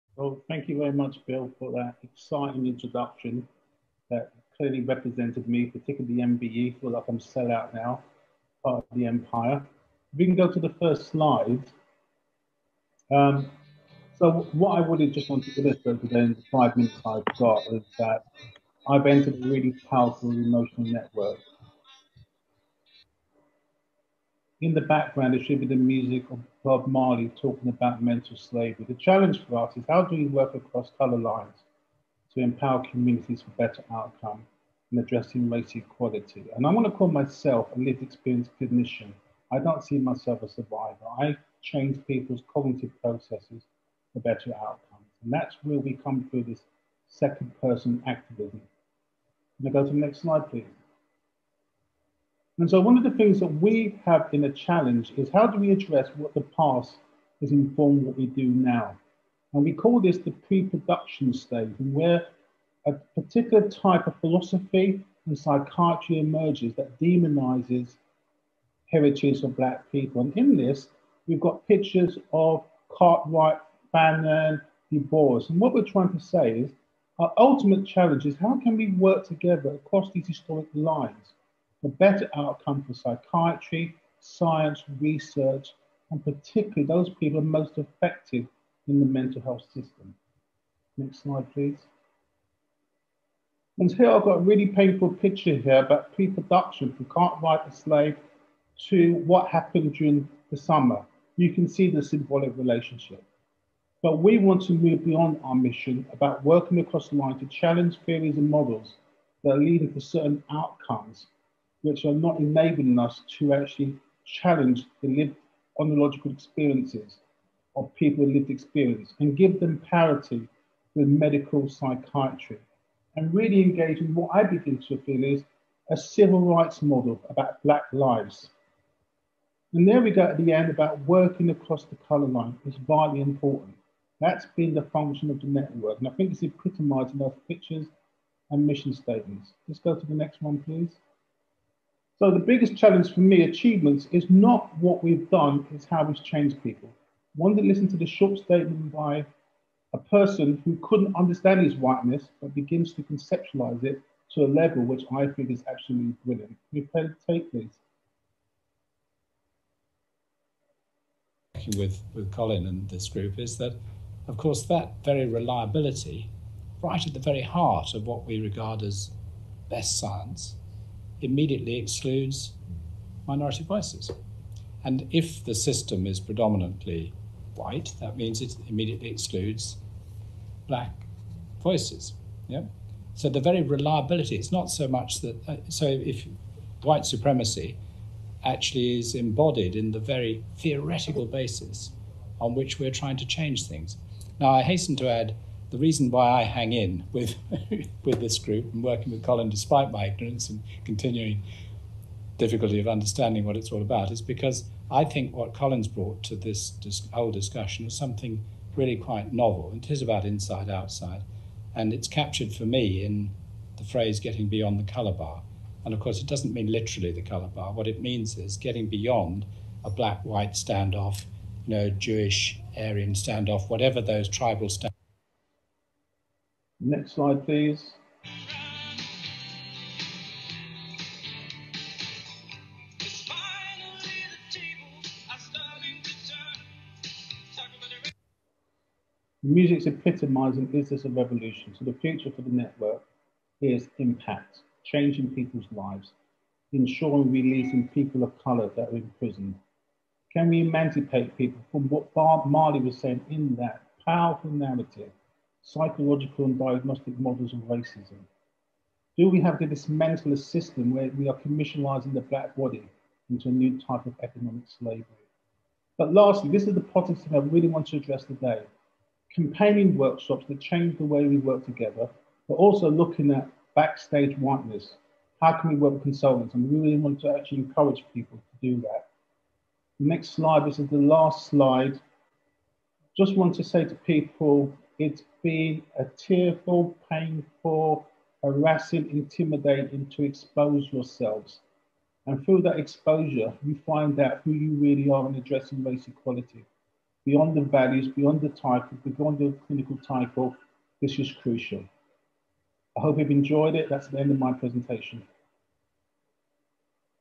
CCVBP 2020: Dr Colin King & Dr Anna Bergqvist - Empowering Communities
Colin King and Anna Bergqvist talk at the 2020 Conference of the Collaborative Center for Values-Based Practice at St. Catherine's College, Oxford

Colin King leads the Whiteness and Race Equality Network lead at the Collaborative Center for Value Based Practice. Colin has a PhD from Goldsmiths and has many other degrees such as MA, MBA, MSc, PGCE. He's recognized with an MBE and is a Hall of Famer for his work a lot to racism in sport.

Anna Bergqvist is an Associate Professor of Philosophy specialising in metaethics (esp. moral perception), philosophy of psychiatry and applied phenomenology concerning self-ownership and relational moral agency. She has published on shared decision-making and cultural values in psychiatry. She is the Director of the Values-based Theory Network at St Catherine’s Collaborating Centre for Values-based Practice at the University of Oxford and Member of the Whiteness and Race Equality Network.